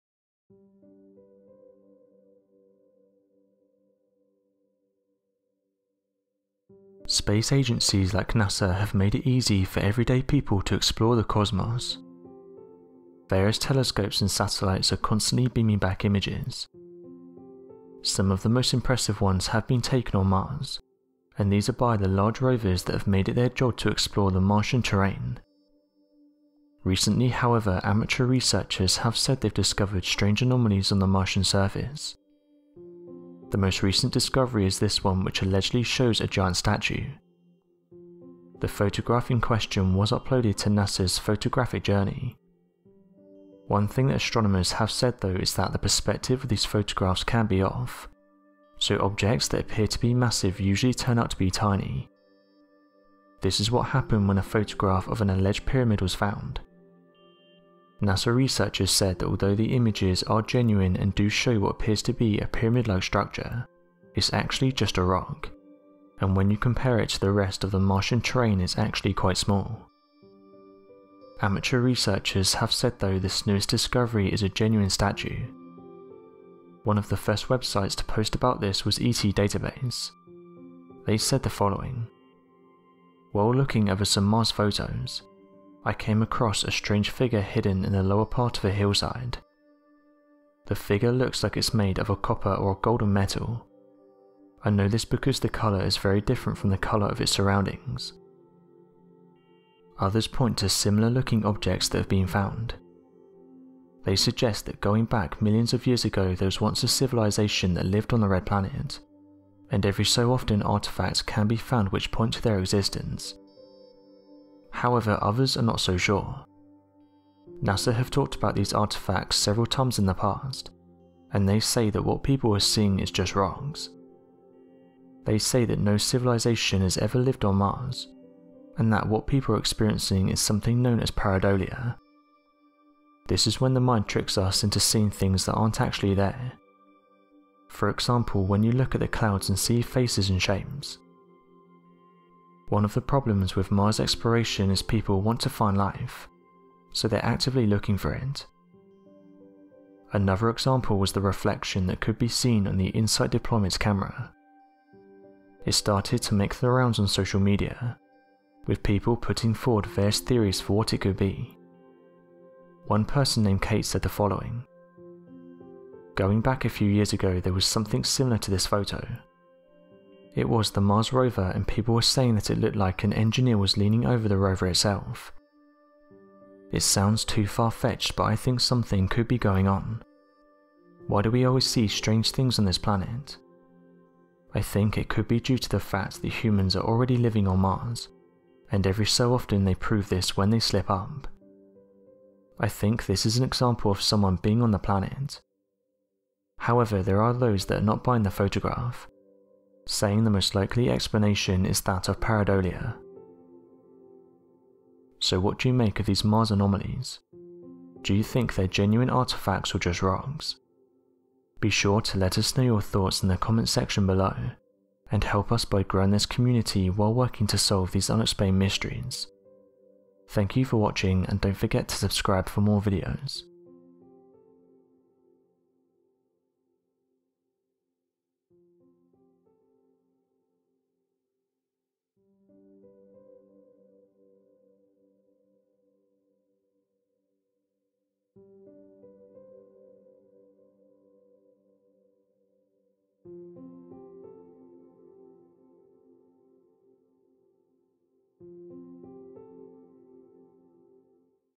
Did NASA Officials Forget To Edit Out This Anomaly On Mars?
Did NASA officials forget to edit out this anomaly on Mars? Today, we take a look at this alleged anomaly that's been discovered on Mars.

Space agencies like NASA have made it easy for everyday people to explore the cosmos. Various telescopes and satellite are constantly beaming back images. Some of the most impressive ones have been taken on Mars, and these are by the large rovers that have made it their job to explore the martian terrain.